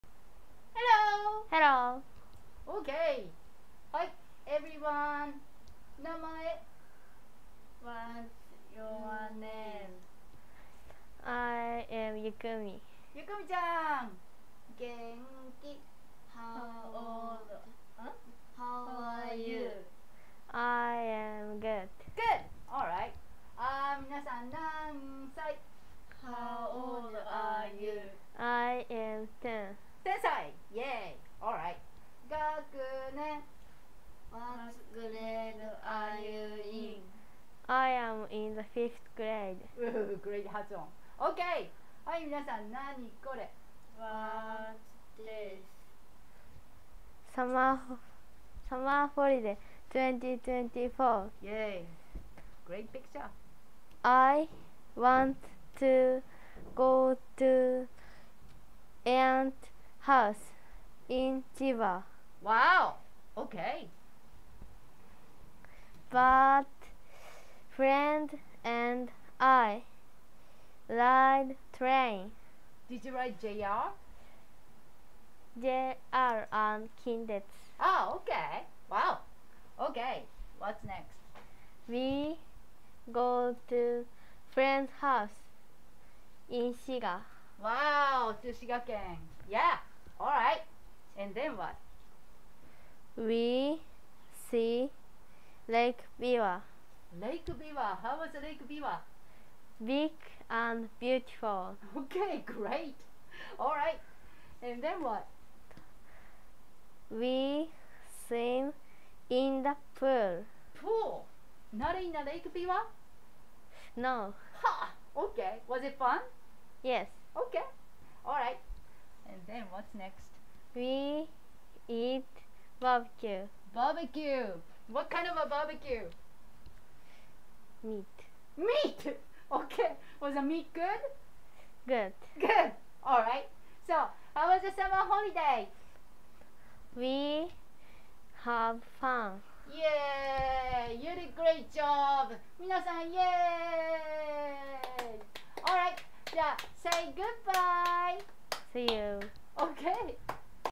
2024 Summer Holiday Yukumi
彼女は小学5年生。
夏休みのお話は2回目。
「ツッコミ」質問もよく理解できています。^ ^;
よく頑張りました。^ ^